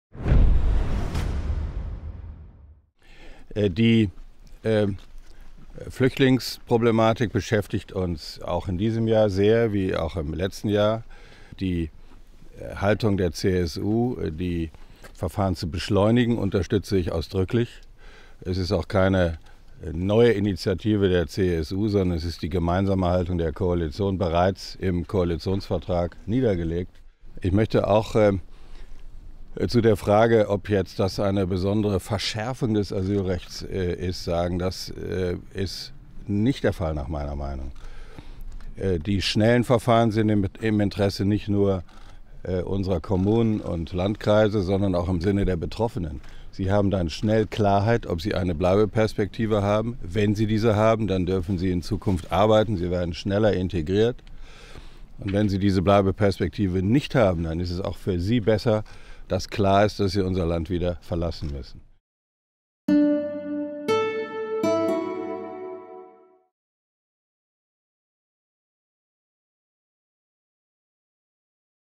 Bundesinnenminister de Maizière zur Asylpolitik
Bundesinnenminister Thomas de Maizière unterstützt die Forderung der CSU nach beschleunigten Asylverfahren  und schnellerer Abschiebung abgelehnter Bewerber.